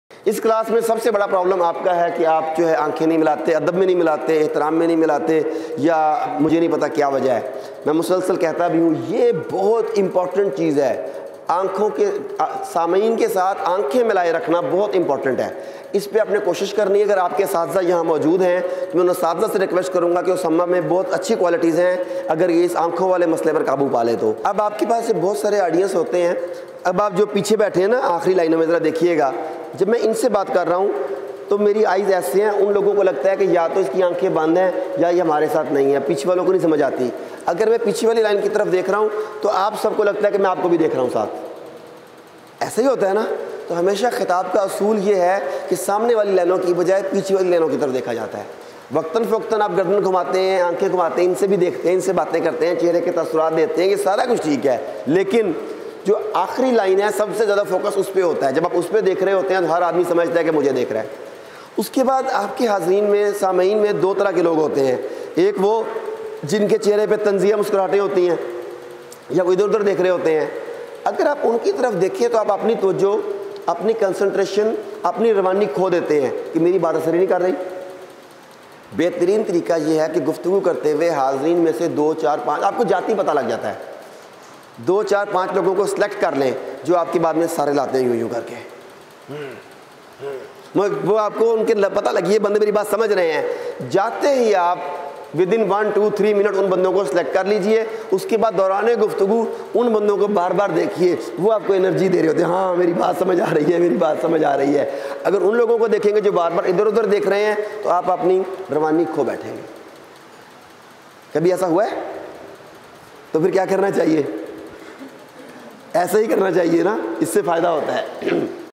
Maintain Eye Contact During Public Speaking || Jamia tur Rasheed || Dr. Arif Siddiqui || Trainer
Public Speaking Workshop Conducted at Jamia Tur Rasheed karachi by Dr Muhammad Arif Siddiqui. It includes Communication skills, Body language, Stage Performance, Handling Questions, Motivational Talks etc.  its unique, matchless, peerless and most exciting and result oriented workshop for Religious Scholars / Ulamma etc.